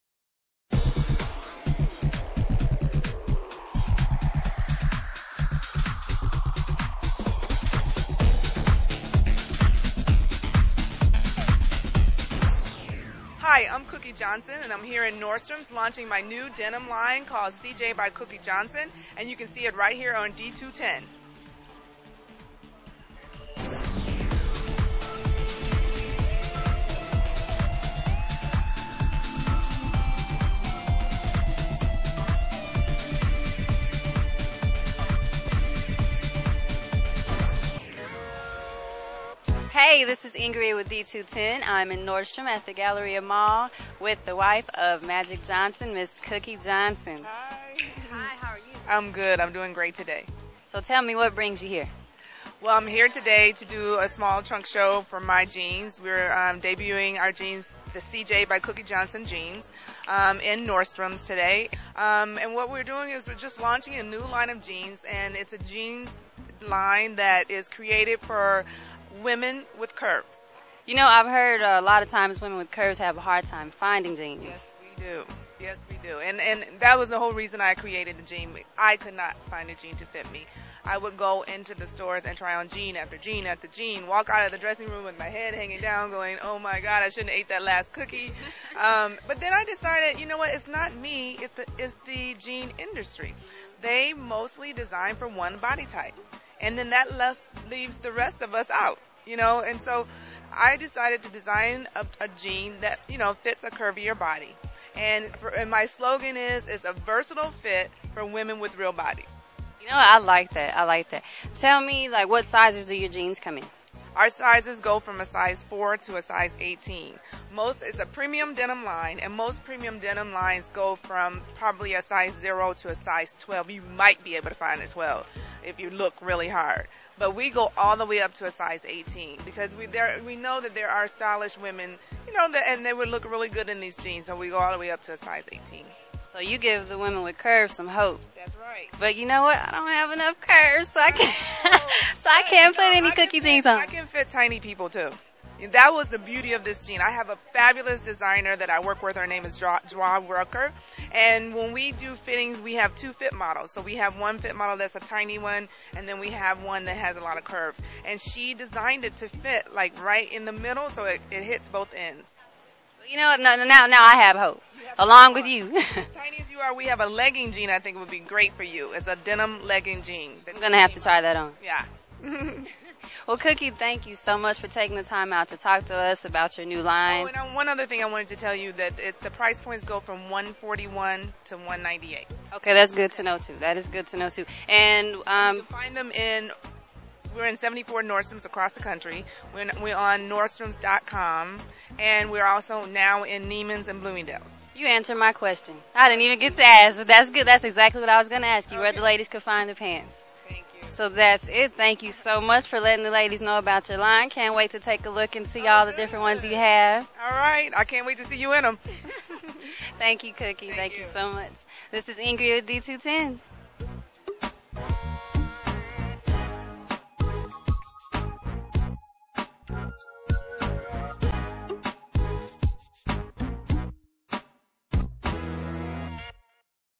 Cookie Johnson Jeans
The jeans are worn by Oprah on a recent cover shoot for her "O" Magazine.
A quote on her website said, "The perfect jeans for real women with real booties..."
The CJ by Cookie Johnson jeans were part of the collection from the basketball star's wife that began in 2009.
Johnson was helped by fashion designer Michael Glasser who was able to find stretchy fabric that made these jeans comfortable even for different sized women.  The waistbands of the jeans are lined with a silk fabric.
The jeans are not for the cheapskates.  Most pairs cost close to the $200 range with some designs close to $100.